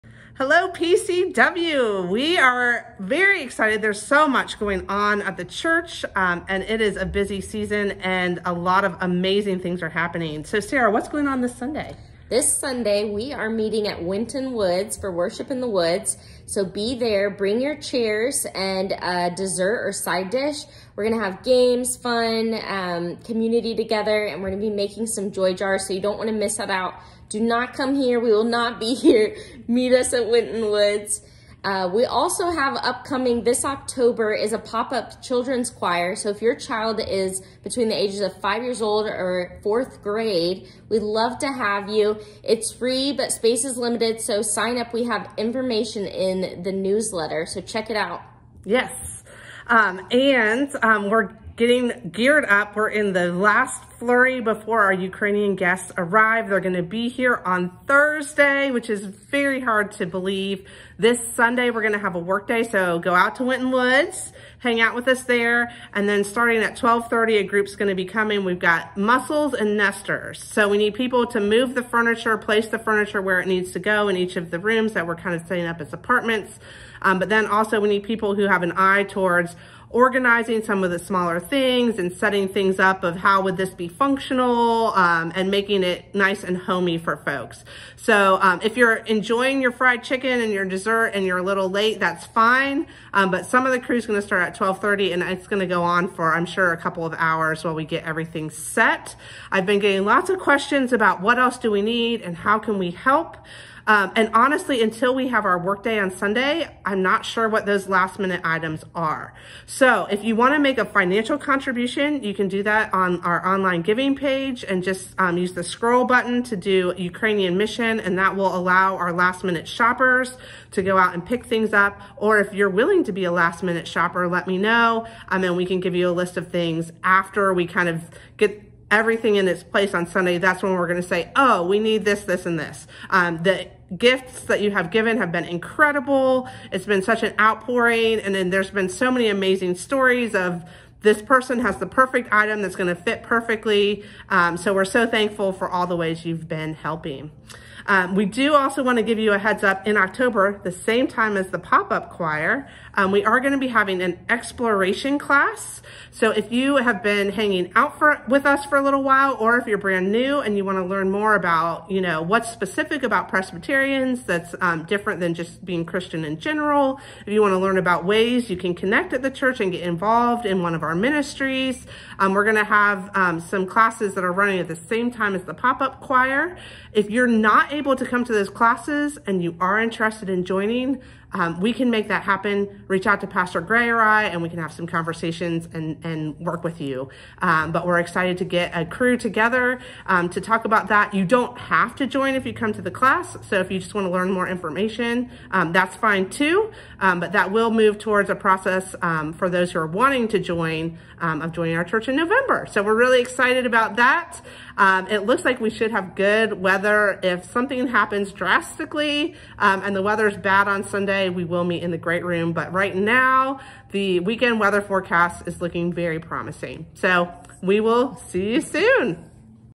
💒❤️ PCW Pastor Update for September 18th 2022
- Worship in the Woods September 18th @ 10am
- Pop up children's choir
- Ukrainian Mission update
- Exploration Class